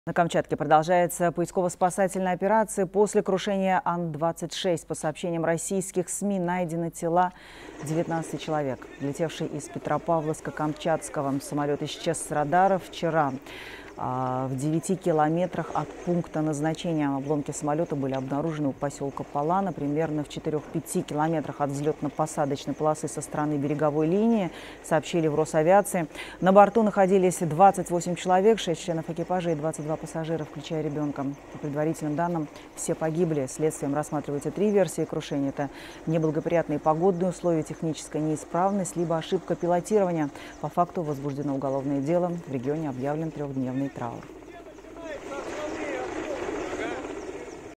На месте крушения Ан-26 на Камчатке обнаружены тела 19 человек
На Камчатке продолжается поисково-спасательная операция после крушения Ан-26. По сообщениям российских СМИ, найдены тела 19 человек. Следствием рассматриваются три версии крушения. Это неблагоприятные погодные условия, техническая неисправность либо ошибка пилотирования. По факту возбуждено уголовное дело. В регионе объявлен трехдневный траур.